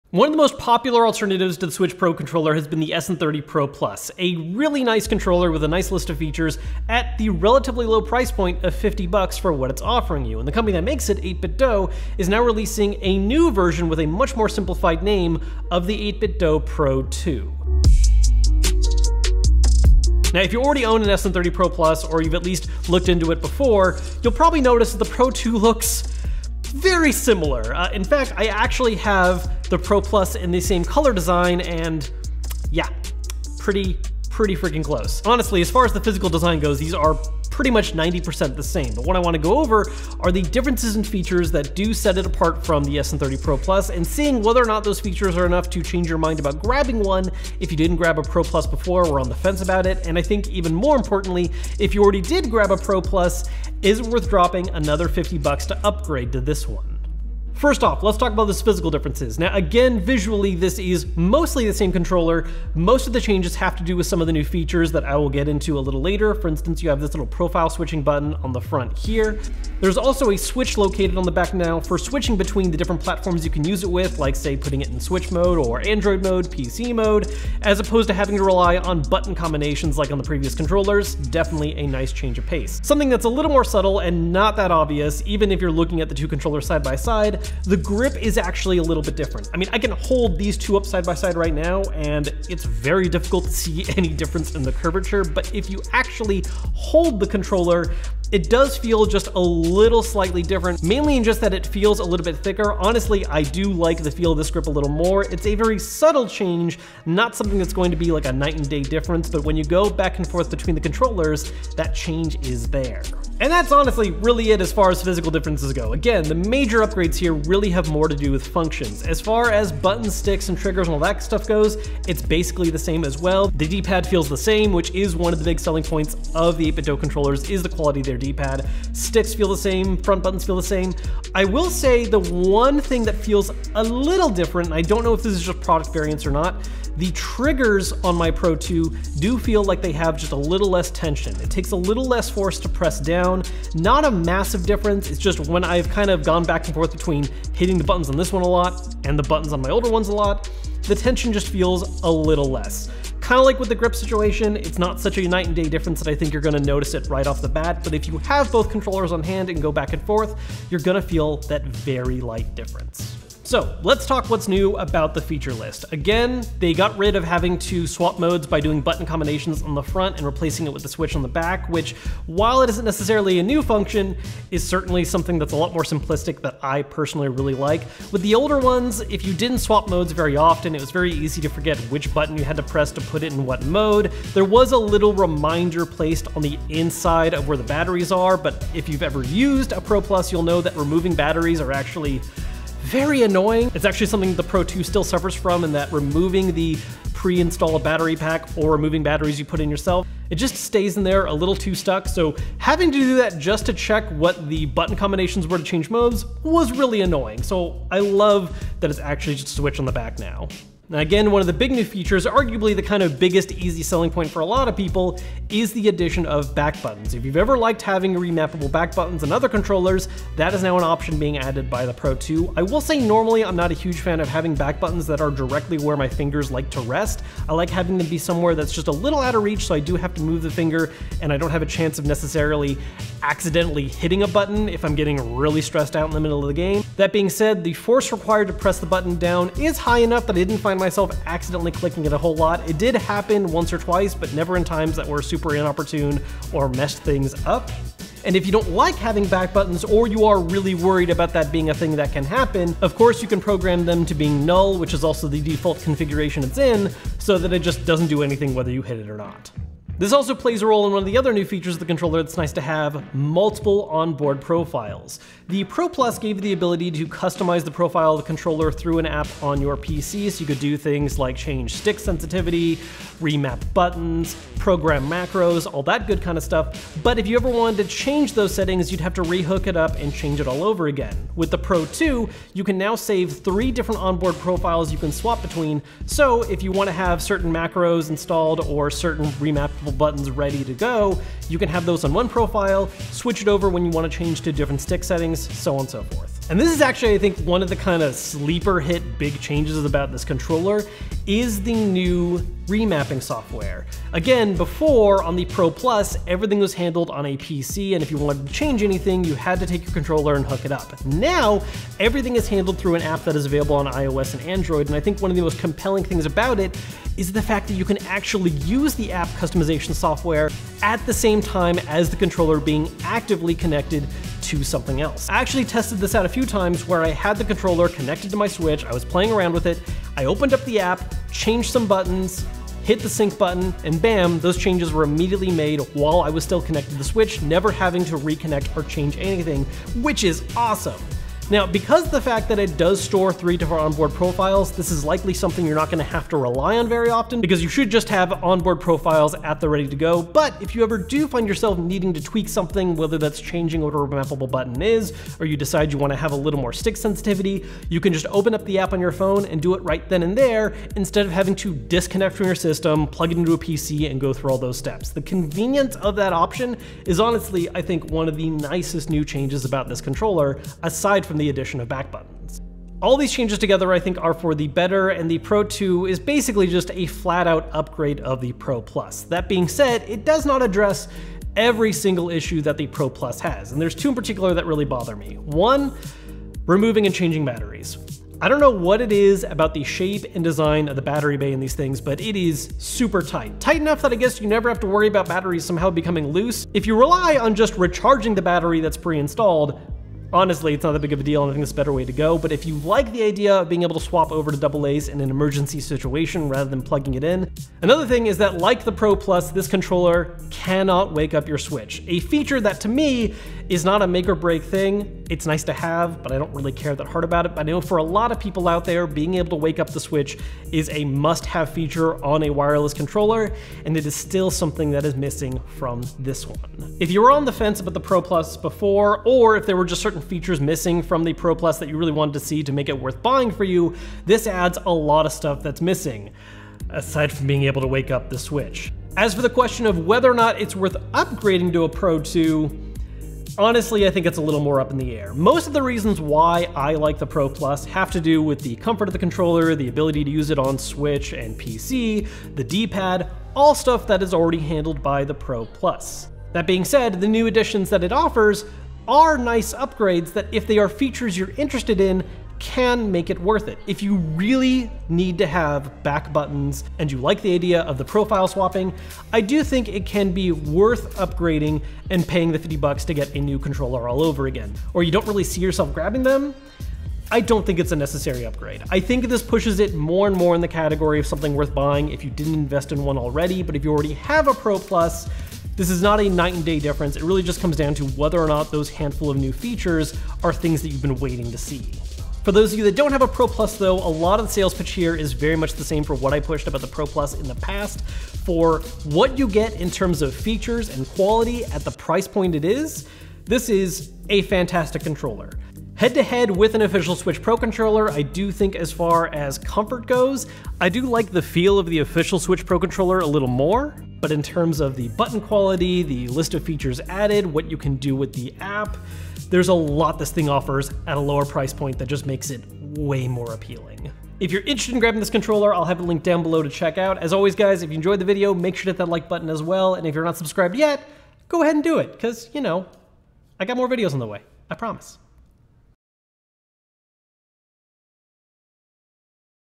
A New Best for Nintendo Switch
One of the most popular Nintendo Switch Pro Controller alternatives just got a brand new version with a list of new features: The 8bitdo Pro 2. Let's talk about what's different and whether or not it's worth upgrading to, or grabbing for the first time!